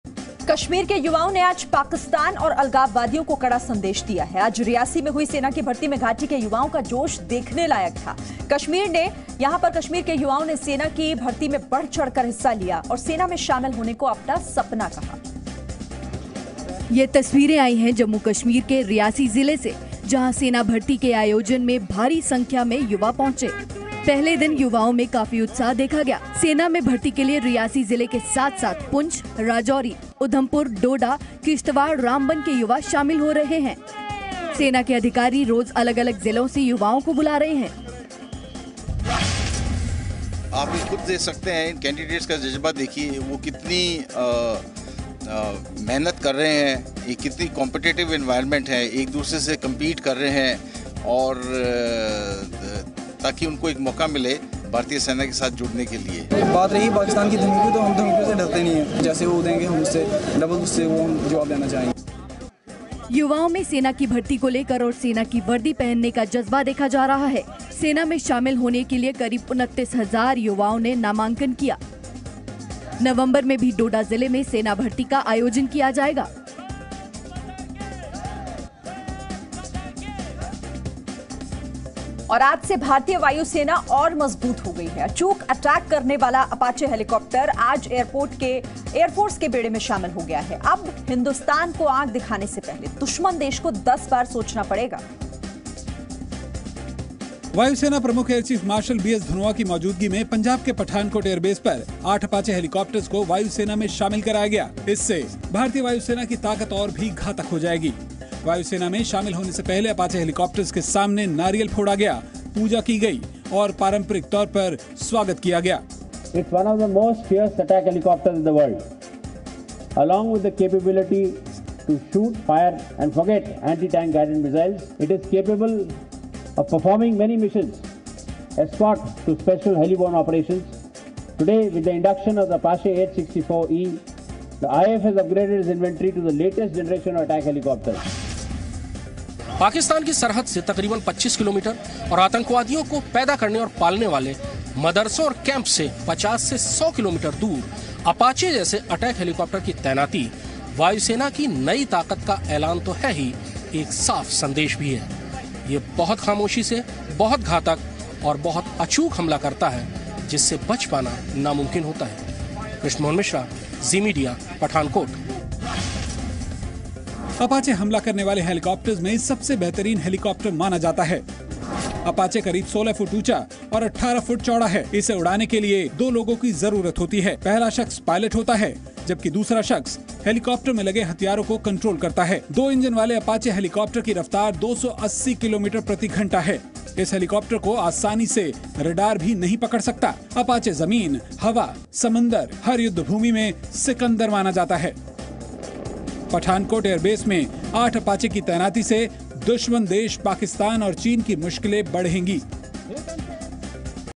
5W1H: Indian Army's recruitment drive attracts Jammu and Kashmir youth
Less than a month after the abrogation of Article 370 in Jammu and Kashmir, the Indian Army on Tuesday organised a recruitment camp at Talwara STC Training Centre in Reasi district of the state